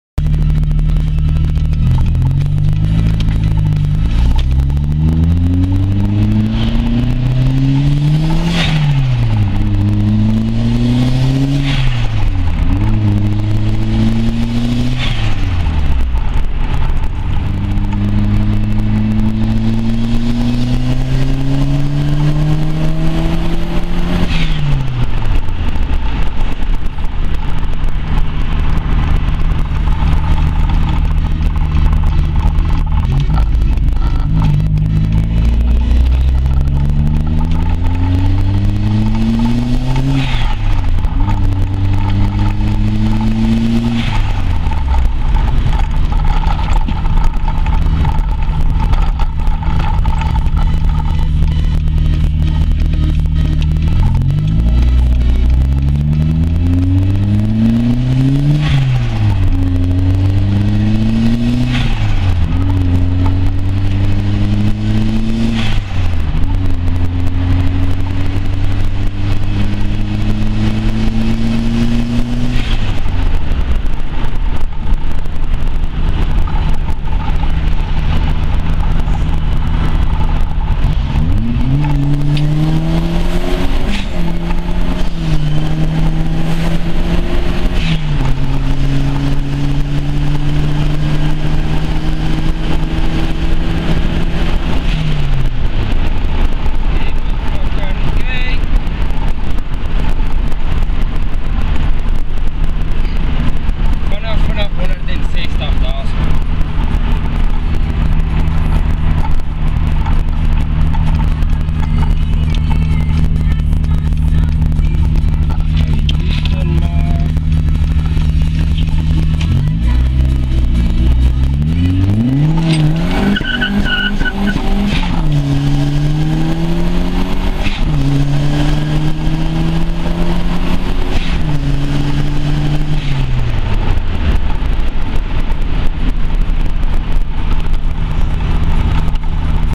130kw golf mk1 1400i 255nm on 0.9Bar JRV
I have been workin on my golf for 3 months now,nd so far getting more kw'ts every time,will upload more videos later on